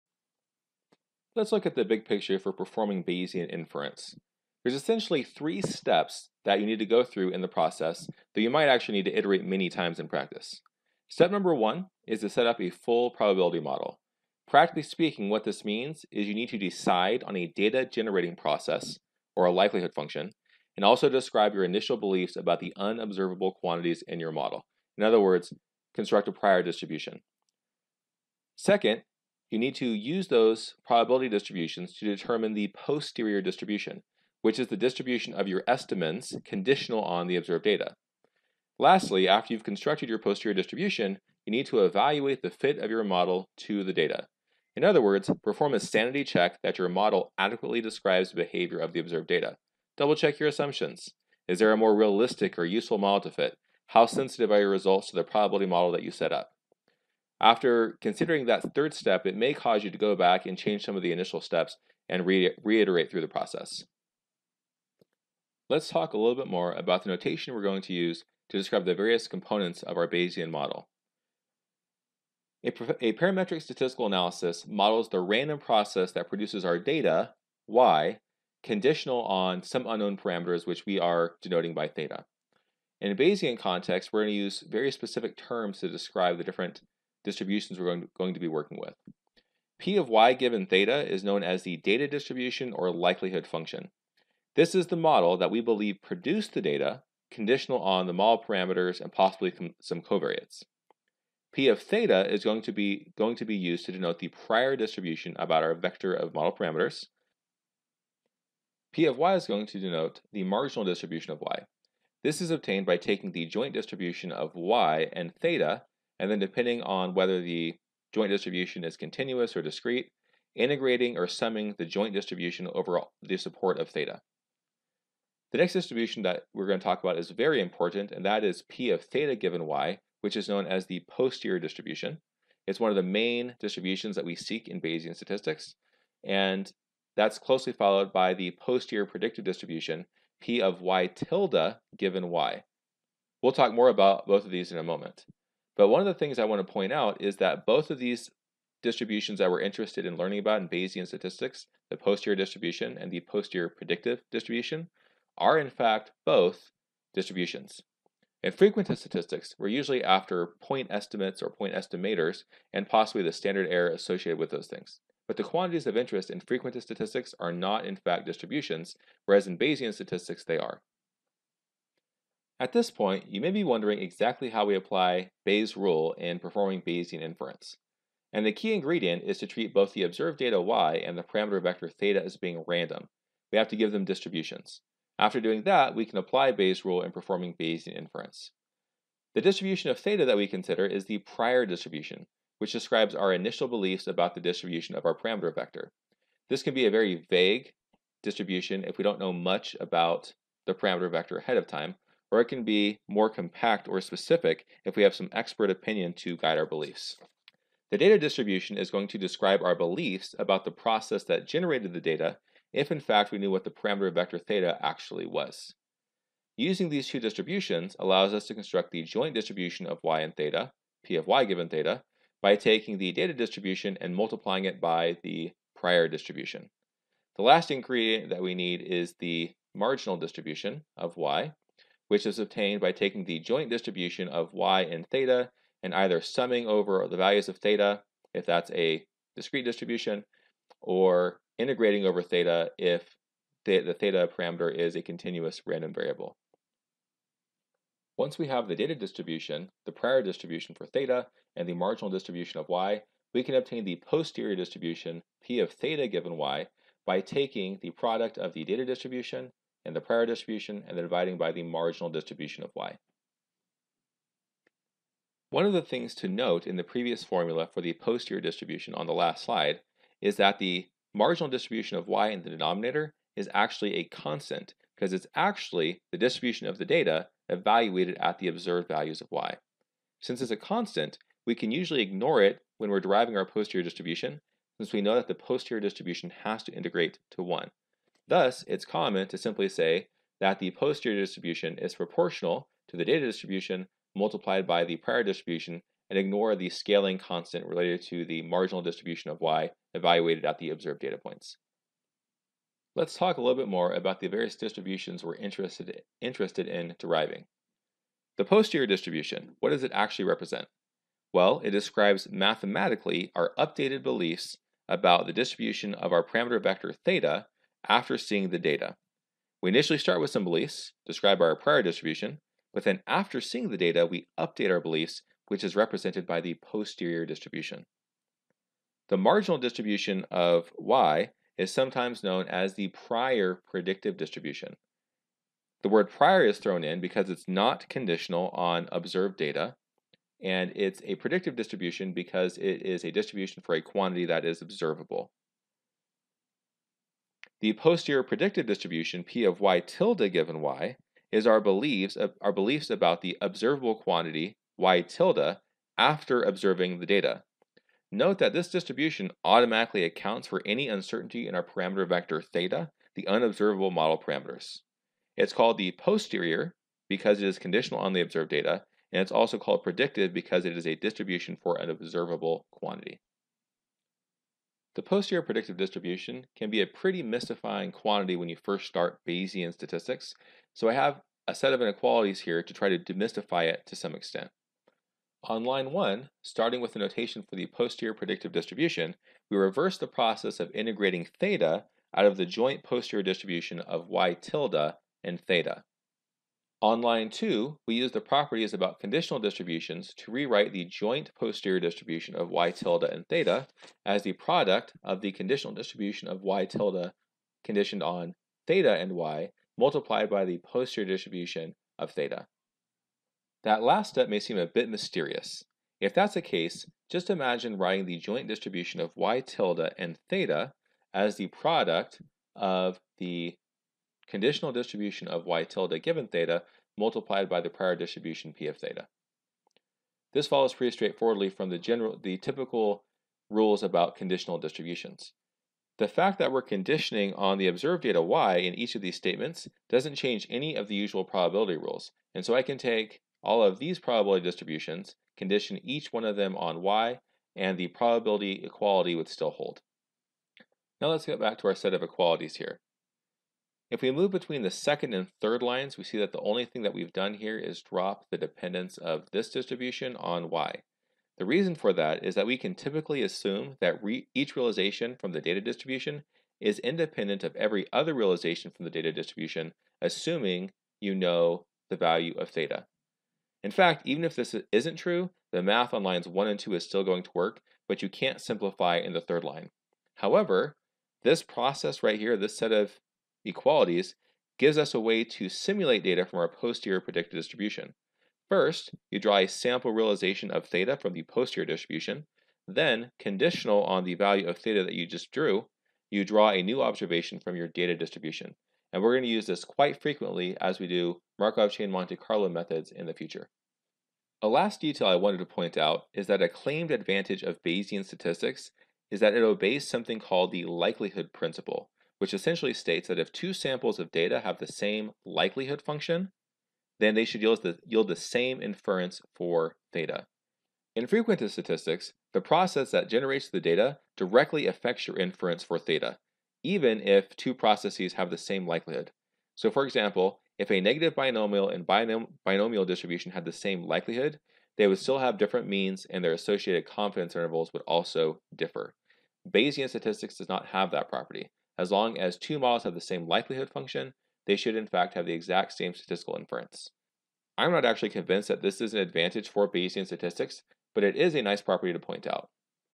Bayesian Statistics - Introduction to Bayesian inference
We discuss some of the key ingredients in performing Bayesian inference.